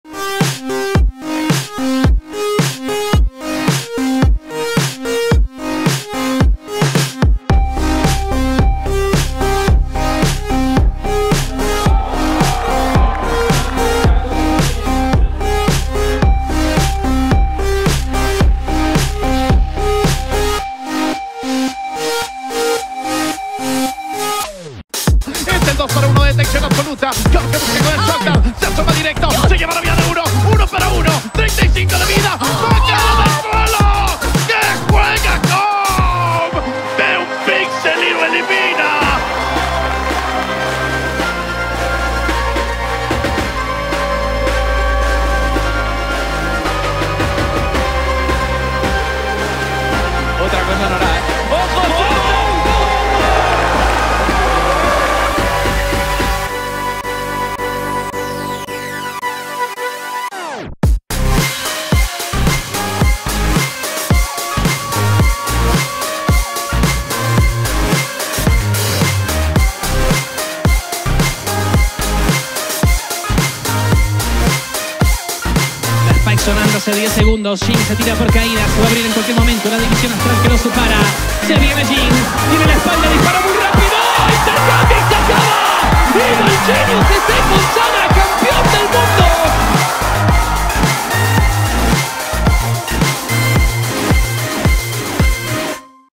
¡Gracias Chile! Así fue nuestra Viewing Party VALORANTChampions | Esports | VALORANT
Leyendas del competitivo, cosplayers de otro mundo, estilazos de la colección Tinnie X Champions, y un público inigualable. Las finales VALORANTChampions se tomaron Chile durante dos días de celebración, repasa en este video lo que vivimos en nuestra Viewing Party.